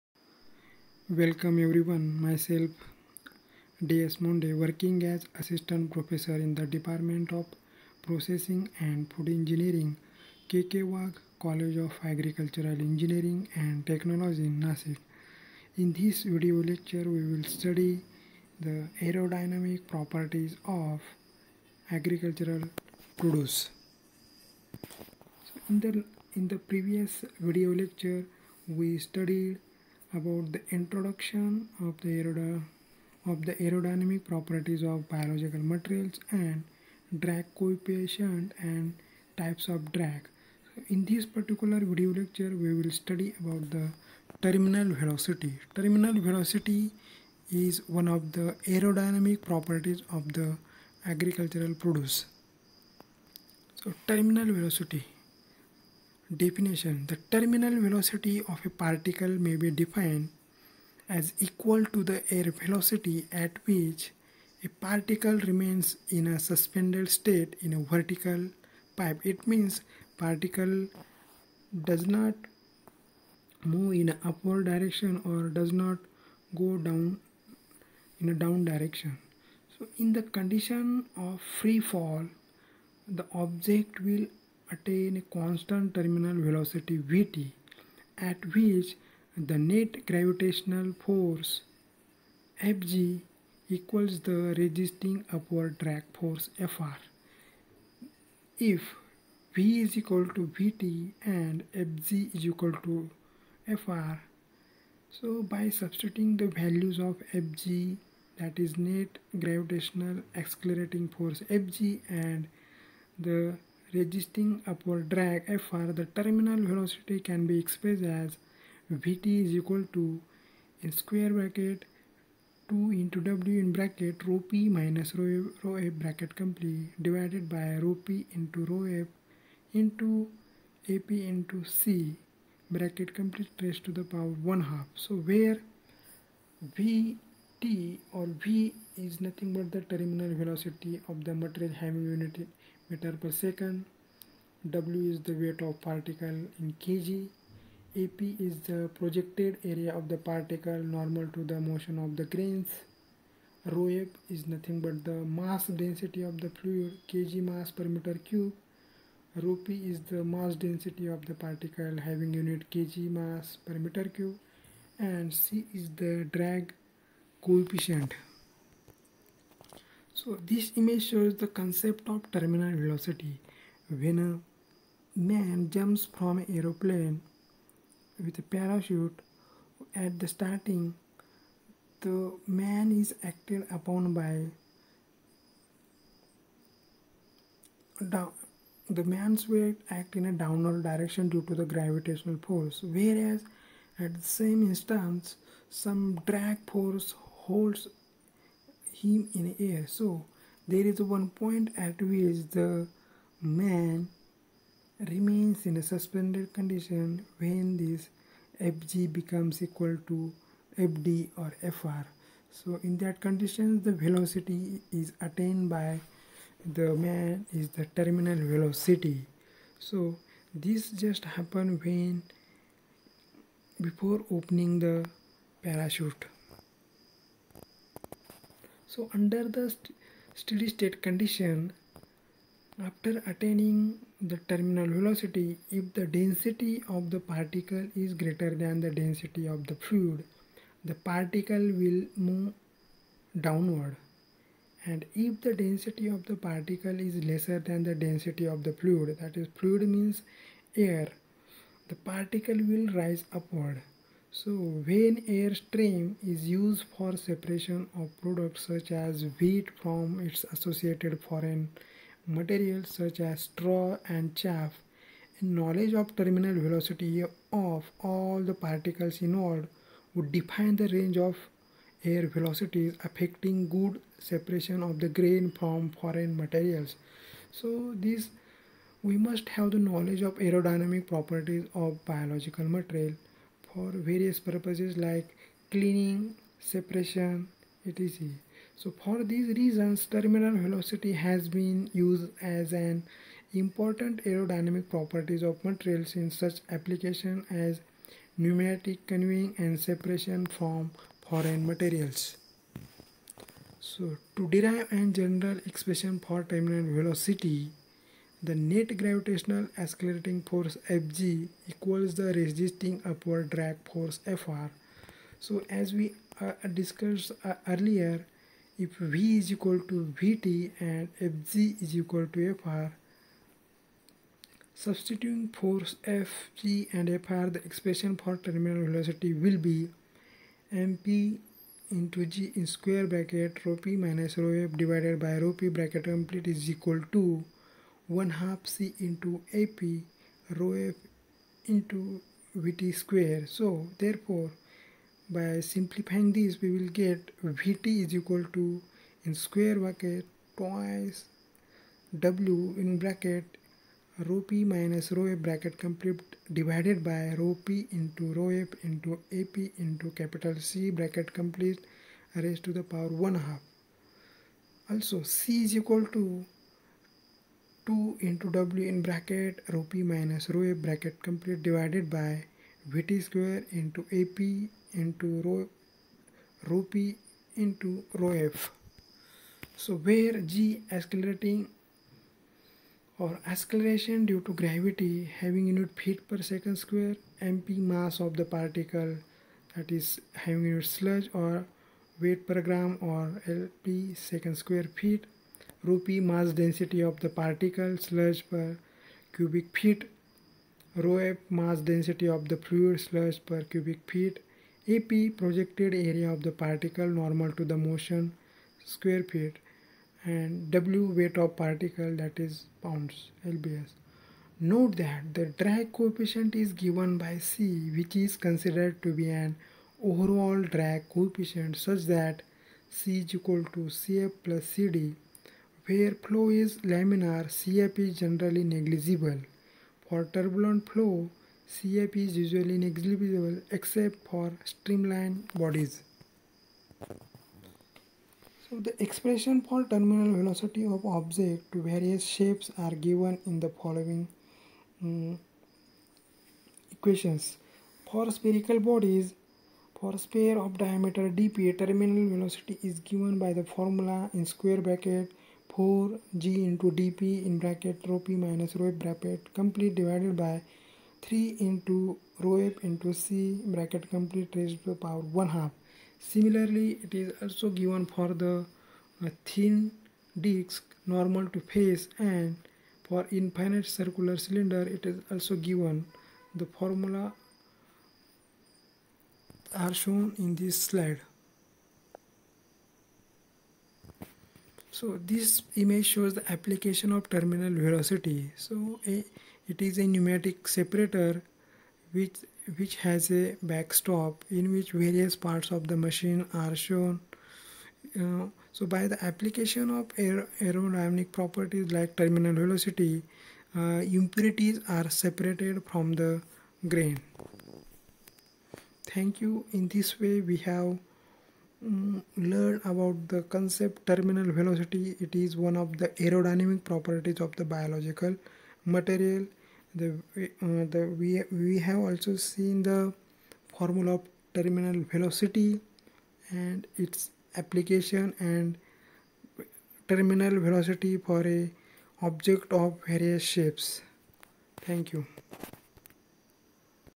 Aerodynamic Properties of Agricultural Produce-II
In this video lecture aerodynamic property of Agricultural Produce such as terminal velocity is discussed in detail. Terminal velocity plays important role in the design of  pneumatic conveying systems and various separators which works on pneumatic properties.